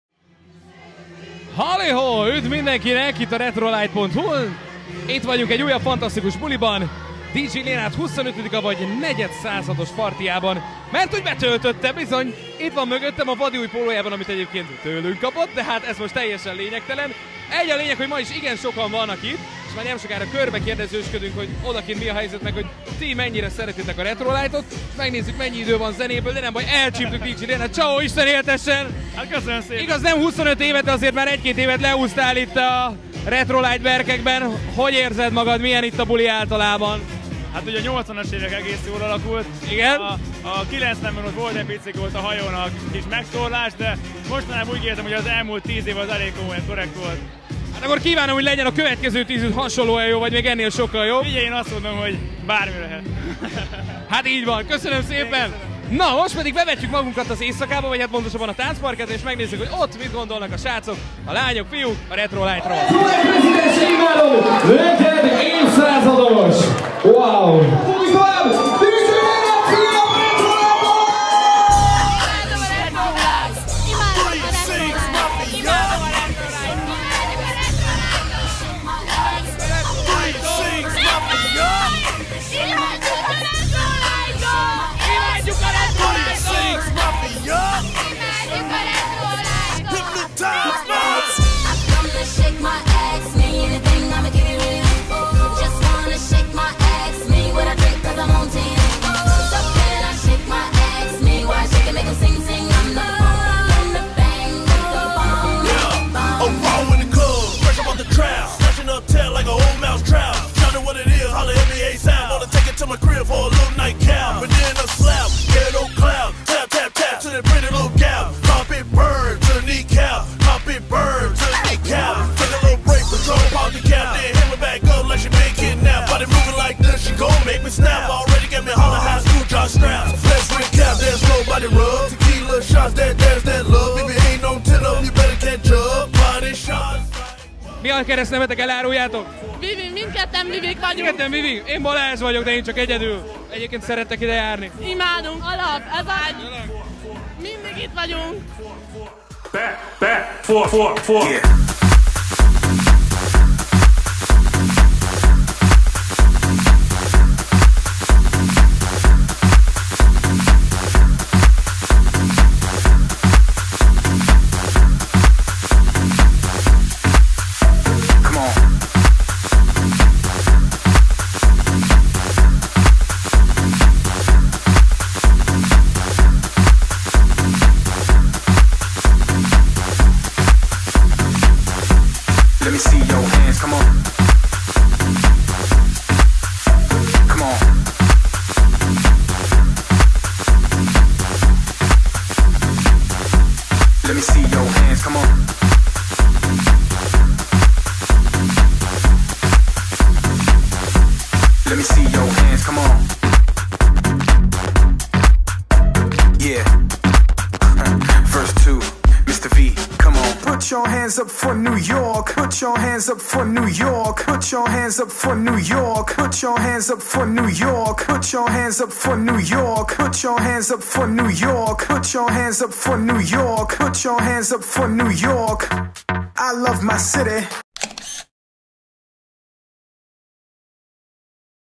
PARTYFILM HD | 10/01/22 RetroLight - Pótszilveszter & Dj Lennard Birthday Party
Fantasztikus hangulatú bulin köszönthettük a RetroLight rezidensét, Lénárdot 25. születésnapja alkalmából - hálásan köszöni, hogy együtt szórakoztatok vele! - emelett pótoltuk a szilvesztert is! :)